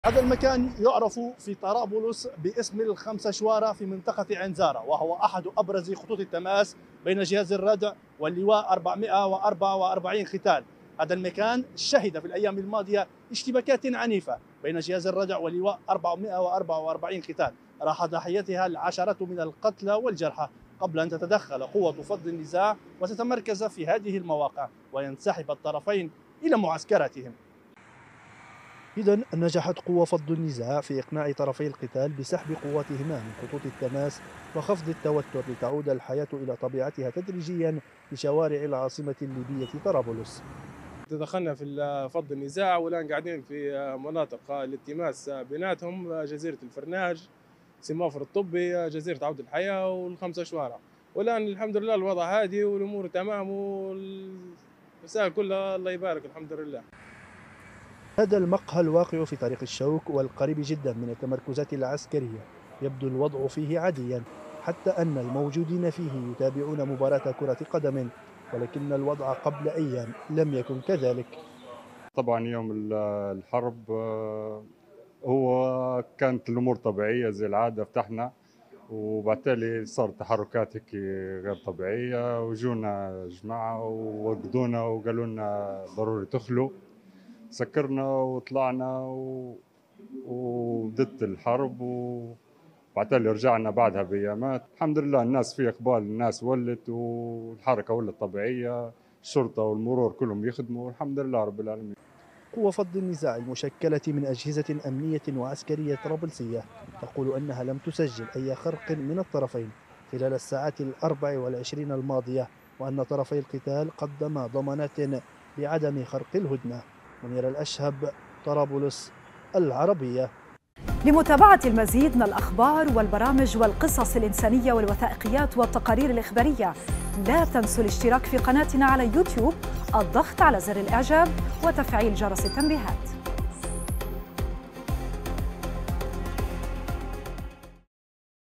كيف نجحت قوة "فض النزاع" في ضبط الأمن وسط العاصمة الليبية؟
قوة "فض النزاع" تنجح في ضبط الأمن وسط العاصمة الليبية.. وطرفا القتال يسحبان قواتهما من خطوط التماس
لمعرفة أحدث المستجدات.. عبر البث المباشر من هنا ⤵
وبإمكانكم متابعة العربية على منصات التواصل